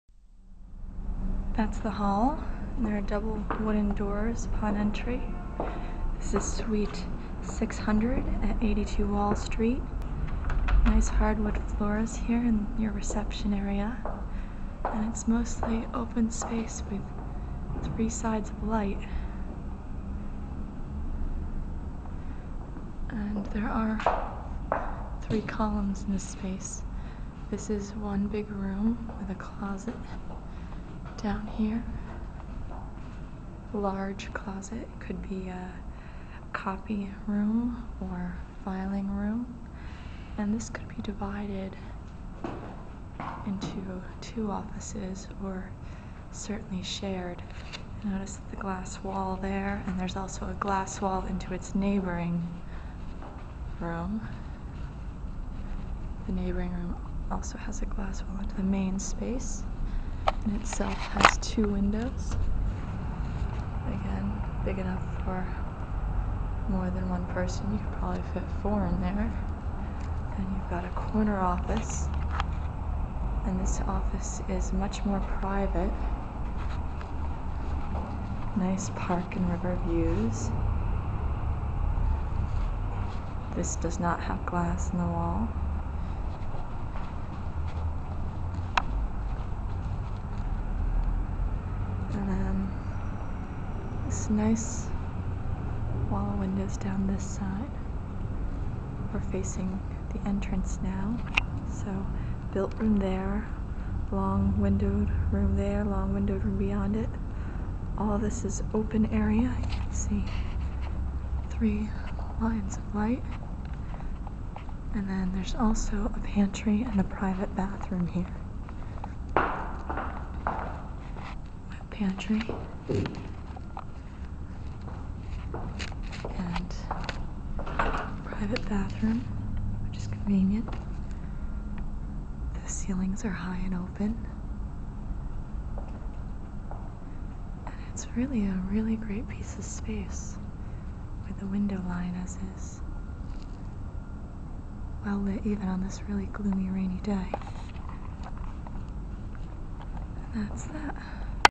326) Wall Street Office : 6fl 3,901sf Asking $26/sf Negotiable
Nice hardwood floors upon entry in reception. Lovely space with three sides of light! Windows completely surround this space.  Private bathroom.  Seperate kitchen with wet pantry.  2 large windowed rooms, windowed into the interior as well, 1 private corner office.  Lots of open space.  Park and river views.

326) Wall Street Office : 6fl 3,901sf Asking $26/sf